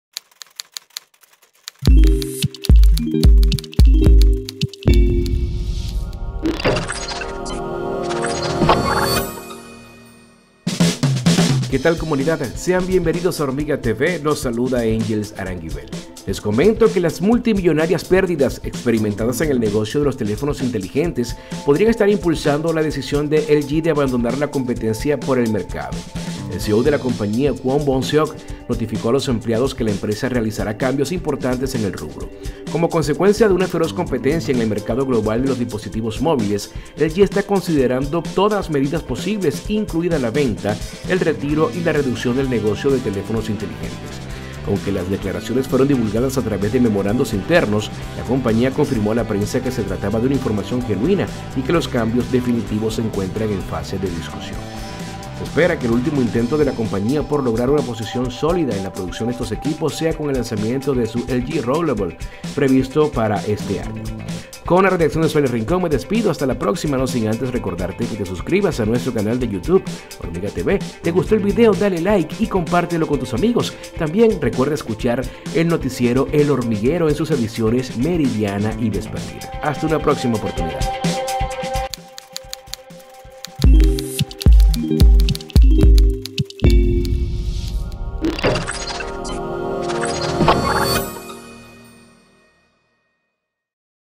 LG CONSIDERA ABANDONAR EL NEGOCIO DE LOS SMARTPHONES | HormigaTV
Las multimillonarias pérdidas experimentadas en el negocio de los teléfonos inteligentes, podrían estar impulsando la decisión de LG, de abandonar la competencia por el mercado.

Se espera que el último intento de la compañía por lograr una posición sólida en la producción de estos equipos, sea con el lanzamiento de su LG Rollable previsto para este año.